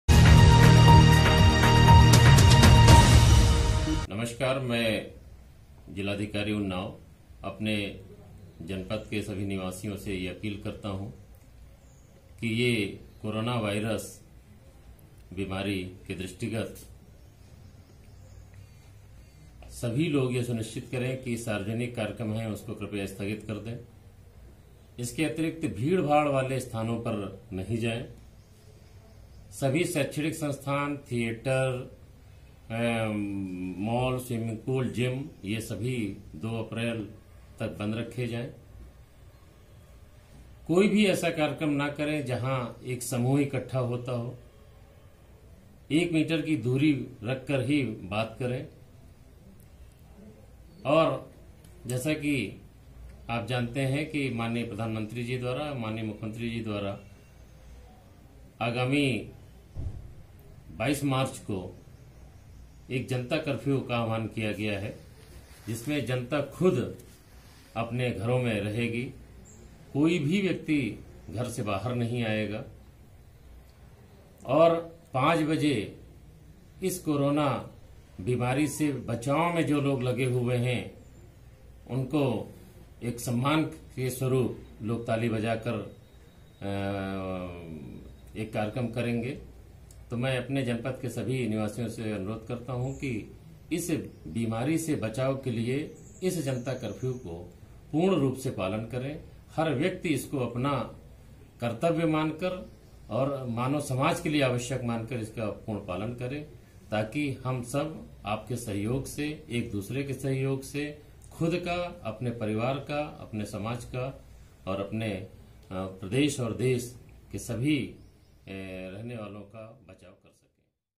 उन्नाव डीएम ने कोरोना वायरस पर जिले की जनता से अपील द्वारा दिया संदेश 22मार्च को घर से ना निकले बाहर
उन्नाव डीएम ने कोरोना वायरस पर जिले की जनता से अपील द्वारा दिया संदेश 22 मार्च को घर से ना निकले बाहर

प्रदेश मीडिया प्रभारी क्राइम रामशरण कटियार की रिपोर्ट

उन्नाव जनपद के जिलाधिकारी रवींन्द्र कुमार ने आज दिनांक 21 मार्च 2020 को जनता से अपील करते हुए कहा है। कि आगामी 22 मार्च 2020 को सुबह 7:00 बजे से रात्रि 9:00 बजे तक घर से बाहर कोई भी व्यक्ति किसी भी तरह नहीं निकलेगा। जिससे कोरोना वायरस पर पूरी तरह से नियंत्रण किया जा सके। इसके लिए भारत सरकार,प्रदेश सरकार ने मिलकर इस घातक बीमारी से निजात पाने के लिए जनता से अपील की कि 22 मार्च को जनता कर्फ्यू नाम के द्वारा जनता को घर से बाहर ना निकलने का आवाहन किया है। इस घातक बीमारी से हम सब मिलकर एक संघर्ष कर जनता कर्फ्यू को सफल बनाने के लिए आदेश का पालन करना अति आवश्यक है। जिससे हम आप सपरिवार खुशहाल जिंदगी गुजार सकें। उन्होंने कहा कि 22 मार्च को सभी जिम थिएटर सिनेमा हॉल माल दुकाने पूर्ण रूप से बंद रहेगी। और जिम हिटलर सिनेमा हॉल एवं कार्यक्रम मीटिंग आदि आदि दो अप्रैल तक पूर्ण रूप से बंद रहेंगे।

इस मौके पर उन्नाव पुलिस अधीक्षक विक्रांत वीर सिंह जिलाधिकारी के साथ इस कार्यक्रम में उपस्थित रहे।